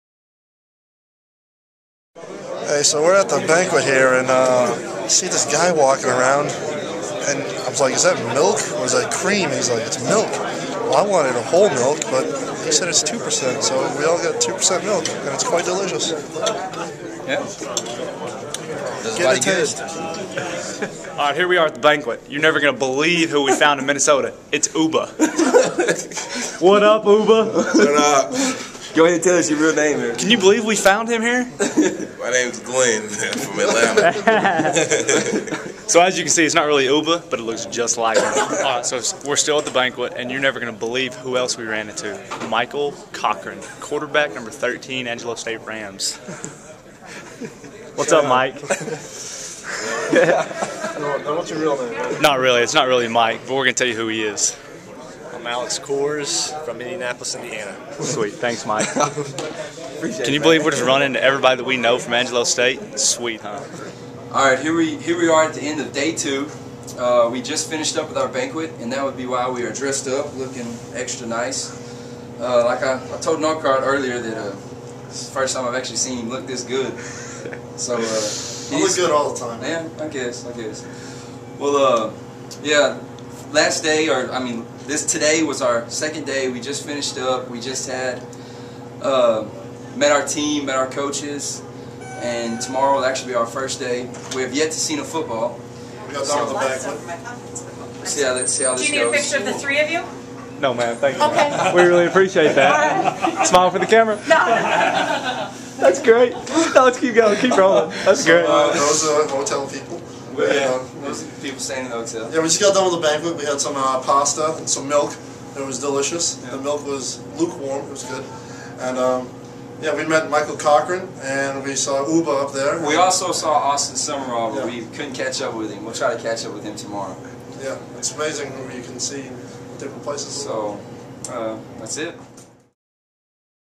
Inside AngeloSports.com: Tackling the Twin Cities - Day Two (12/17/10)
Three Angelo State football players will have a chance to showcase their talents at the All-American Bowl in Minneapolis, Minn., Saturday. Seniors Josh Neiswander, John Norcott and Garrett Tidwell ran into more shenanigans Thursday at the All-American Bowl banquet. AngeloSports.com has a recap of their second day on the road.